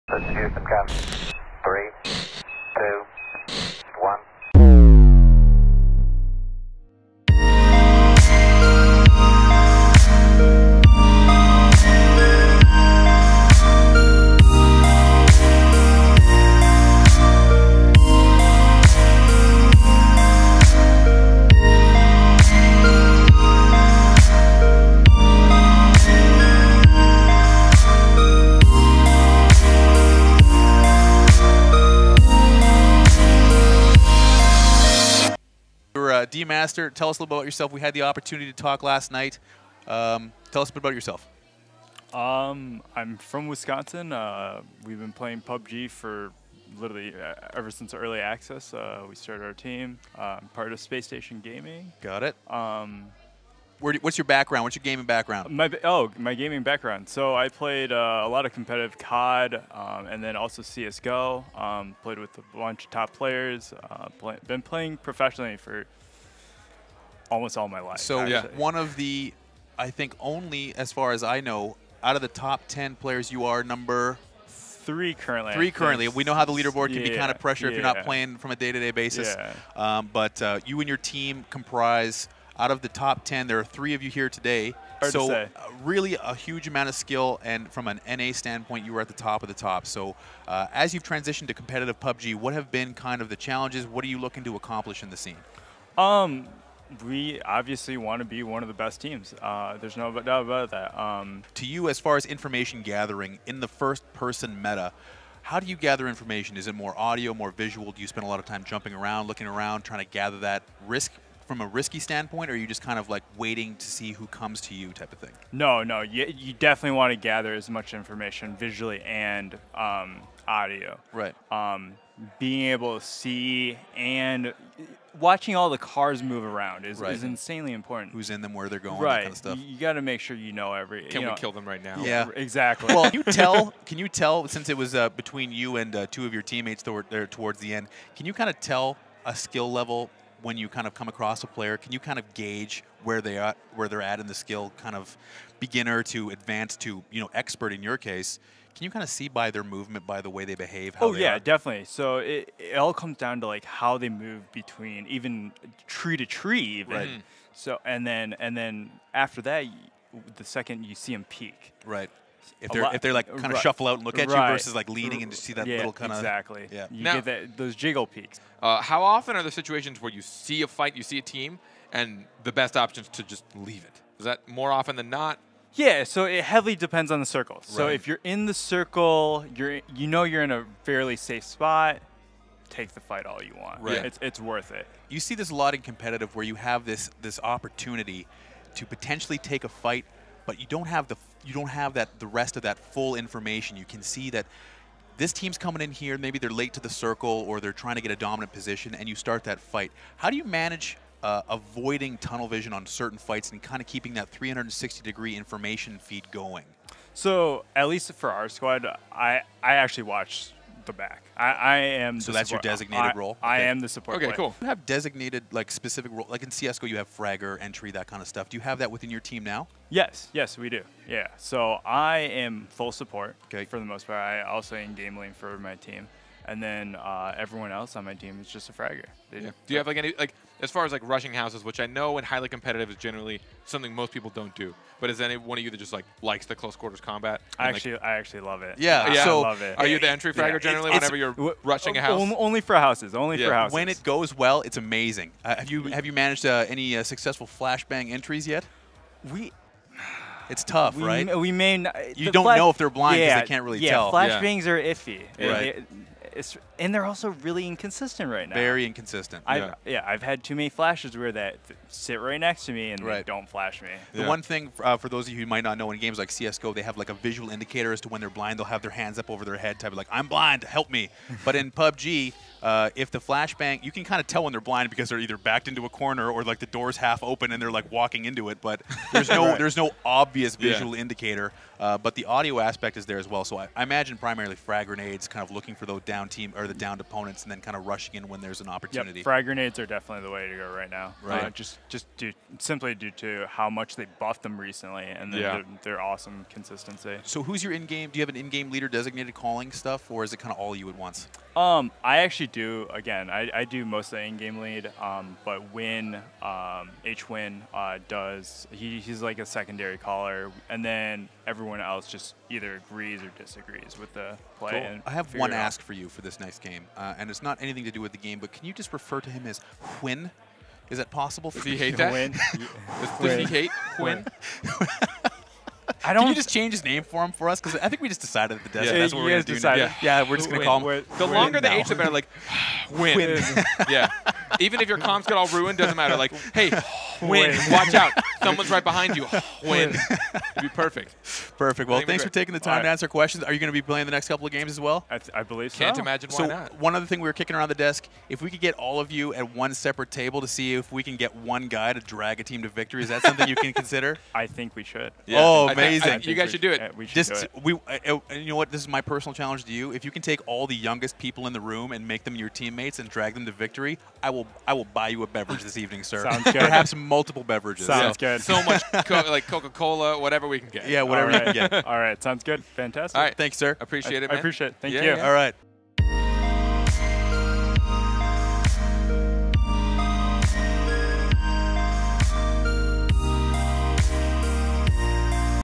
How to dominate PUBG with DmasteR!!
Pro PUBG player DmasteR gives tips and tricks on how to be a better player.

After dominating the competition at the TenGIG Festival tournament DmasteR from Spacestation Gaming gives his thoughts on competition, pro play, esports, and how to be a smarter and better player.